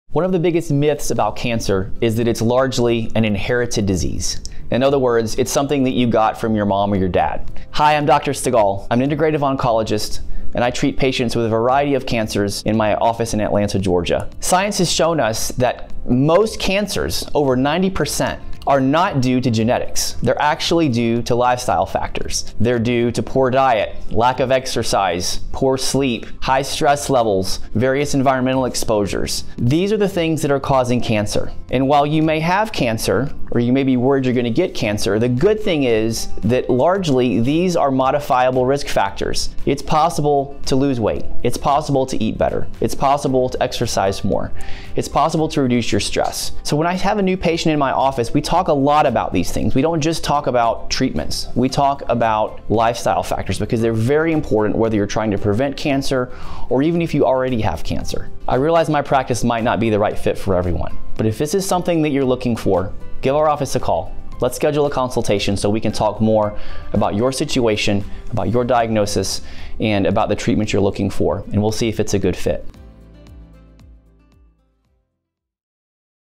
Debunking the Myth: Is Cancer Mostly Inherited? A Lifestyle vs. Genetics Discussion | Dr. Stegall
Is cancer an inherited disease passed down from your parents? Dr. Stegall, an integrative oncologist, dispels this common myth and explores how lifestyle factors play a crucial role in the occurrence of cancer.

What's Covered in This Video:
The misconception that cancer is primarily an inherited genetic disease
Scientific evidence showing that over 90% of cancers are due to lifestyle factors such as diet, exercise, stress, and environmental exposures
The importance of modifiable risk factors and how changes in lifestyle can affect your cancer risk
How our integrative approach considers lifestyle factors in both cancer treatment and prevention

Don't be held captive by myths. In our practice, we focus not just on treatments but also on lifestyle changes that can significantly impact your cancer journey—whether you're looking to prevent it or already diagnosed.